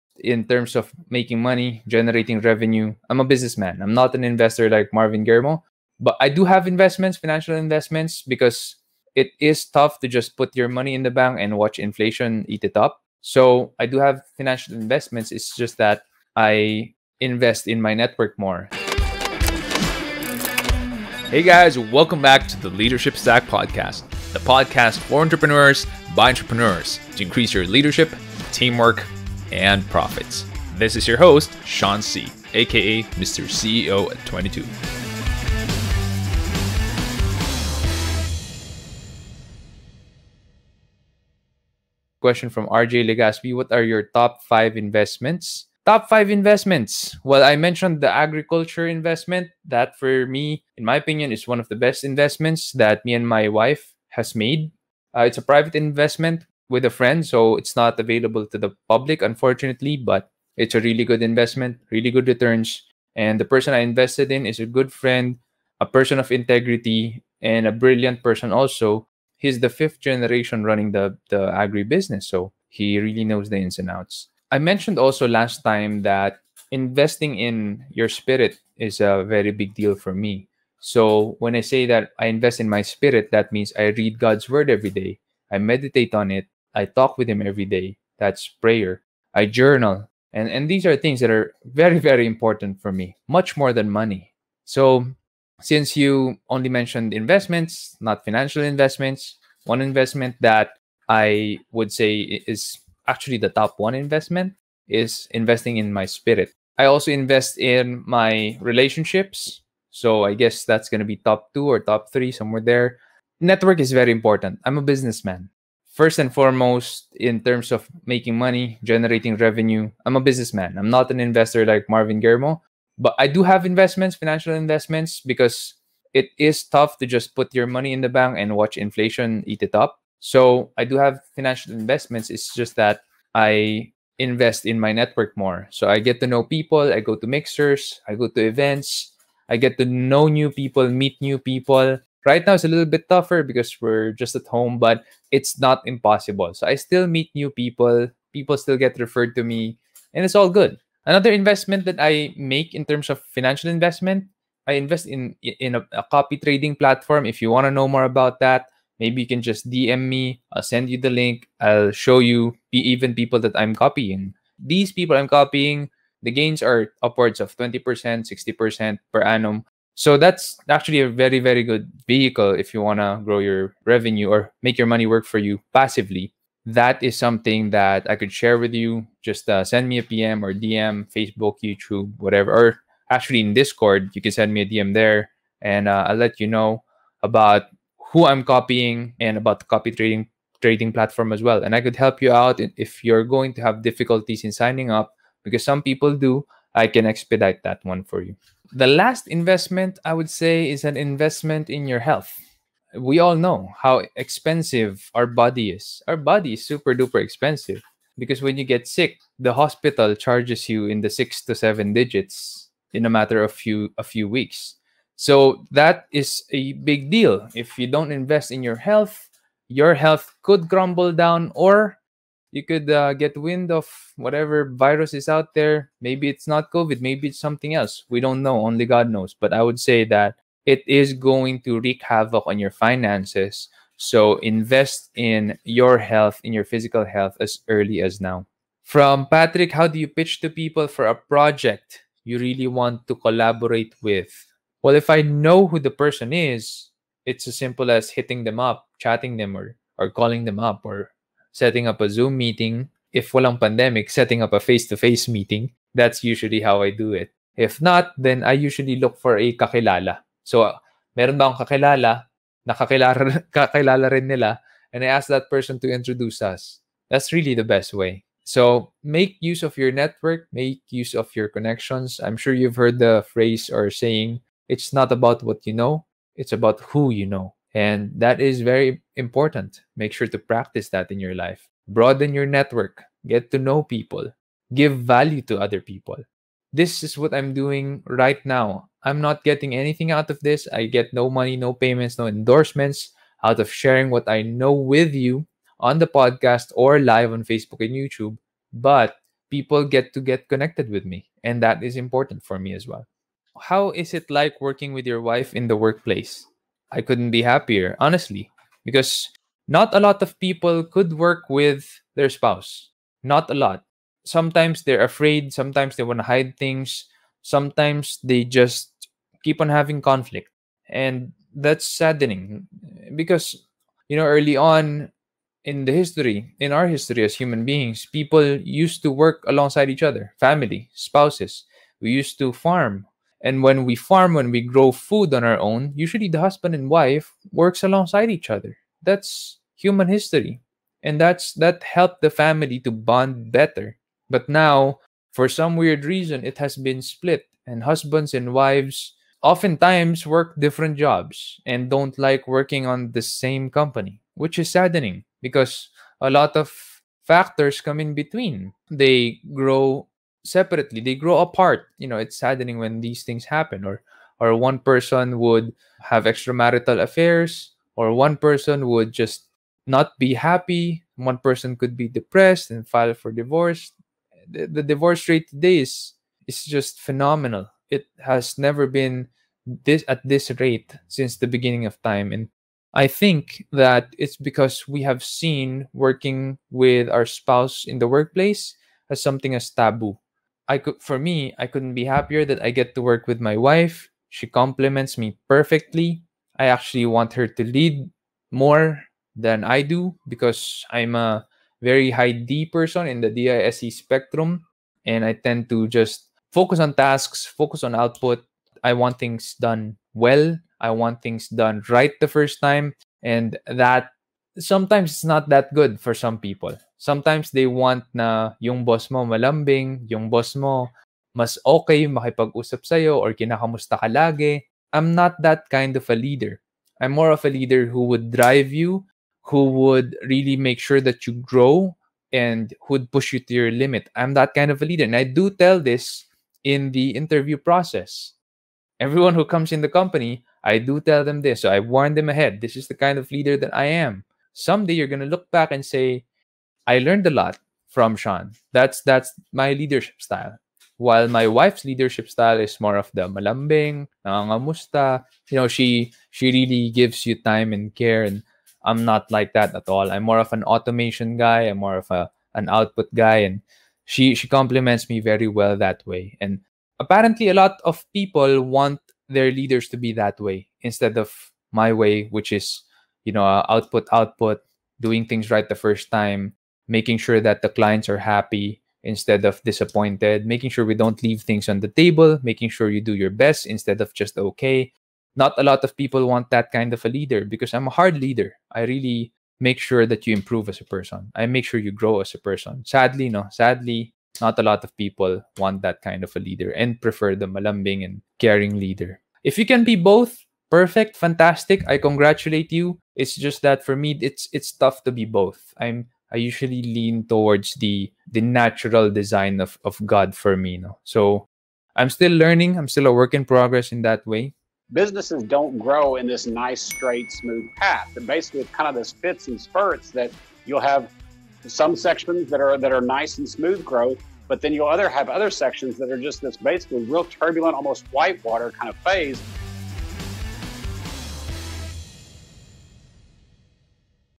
The Five Best Investments For Entrepreneurs (AMA #27)
Want to know about the best investments for entrepreneurs? Learn about the five investments you should be making right now with Mr. CEO at 22 himself, Sean Si. Tune in to learn more about different leadership styles in the workplace as well. All in this jam-packed episode of Ask Me Anything!

Timestamp
0:00 - Introduction
0:41 - Top Investment In Philippines
3:58 - How To Bid For A Project
5:03 - Working With A Family Member
8:35 - Outro